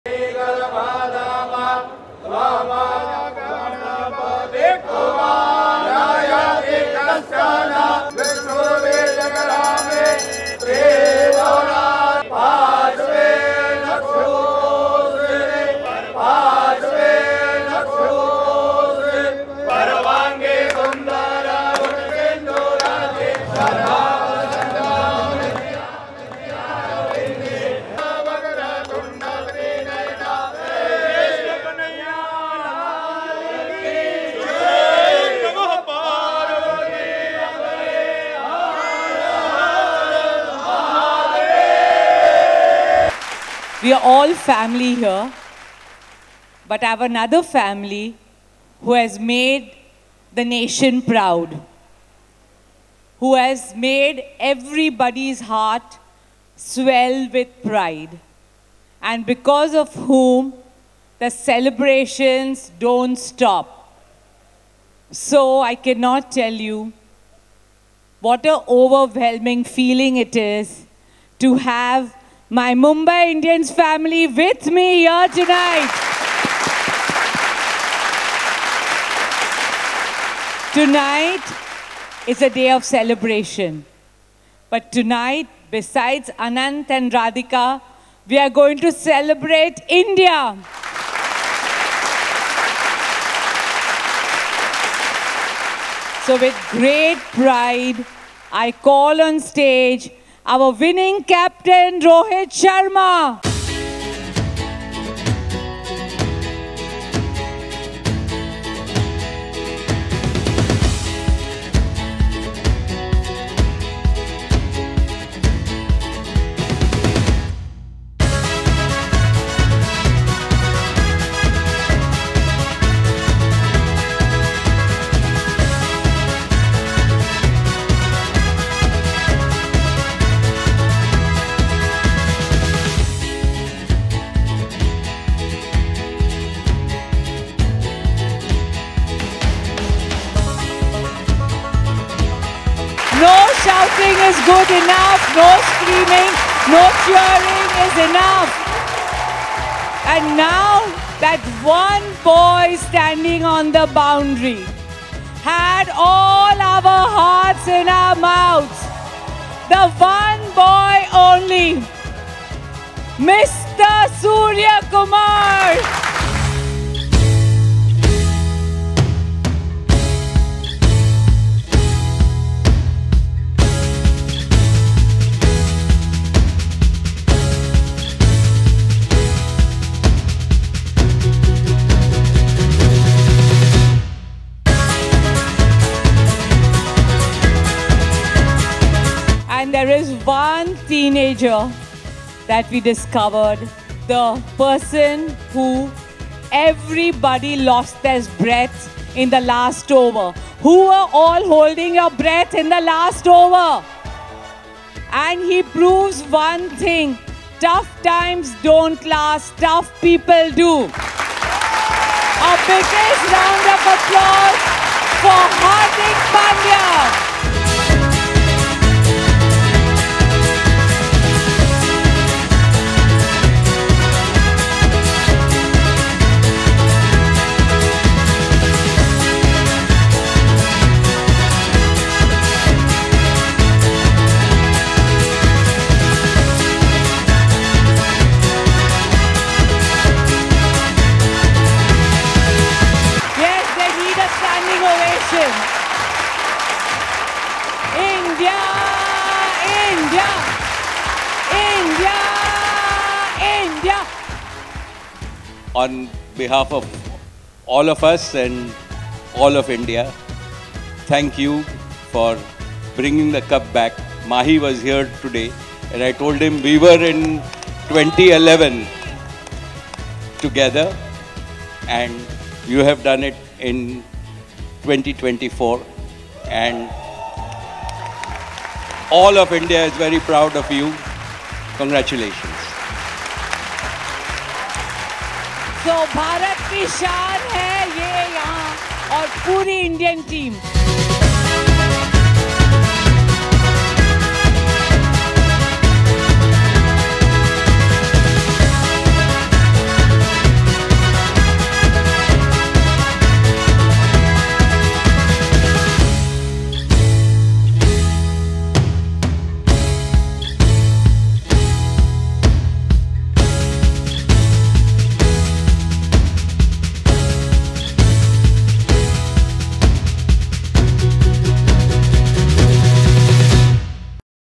Indian Cricket Players honored by Ambani Family : অনন্ত ৰাধিকাৰ বিয়াত বিশেষ অতিথি | N18V
Indian Cricket Players honored by Ambani Family | অনন্ত আম্বানি আৰু ৰাধিকাৰ বহুচৰ্চিত বিয়াৰ সংগীত সমাৰোহত চামিল হ’ল বিশষ অতিথি। পুষ্পবৃষ্টি আৰু তিলকেৰে  T20 বিশ্বকাপ জয়ী ভাৰতীয় দলৰ অধিনায়ক ৰোহিত শৰ্মাৰ লগতে সূৰ্য কুমাৰ যাদৱ আৰু হাৰ্দিক পাণ্ডিয়াক আদৰণি জনালে আম্বানি পৰিয়ালে।
লগতে চাওঁক দিনটোৰ বহু গুৰুত্বপূৰ্ণ খবৰ |